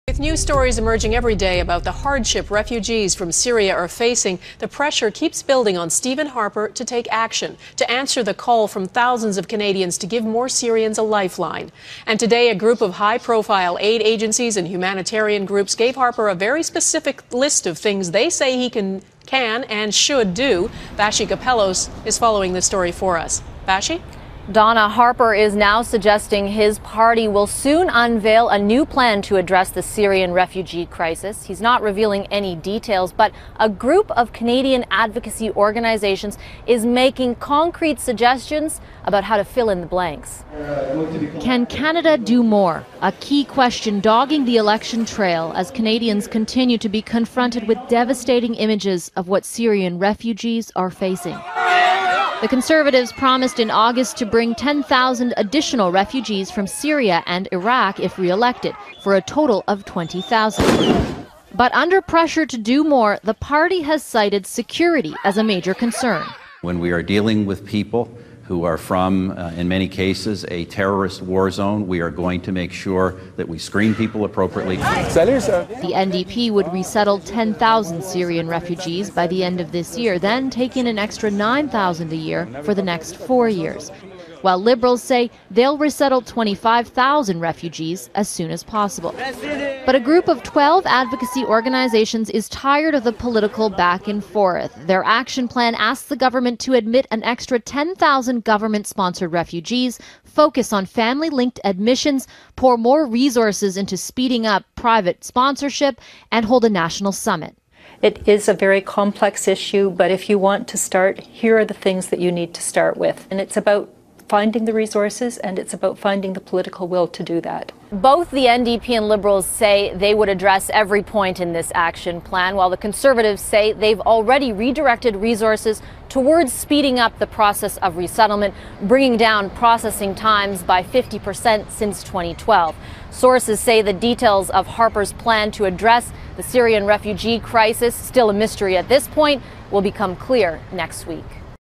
Pressure keeps building for Stephen Harper to do more for Syrian refugees
The pressure for Prime Minister Stephen Harper keeps building for him to do more to help refugees fleeing Syria, including aid agencies and humanitarian groups giving Harper a list of things he should to do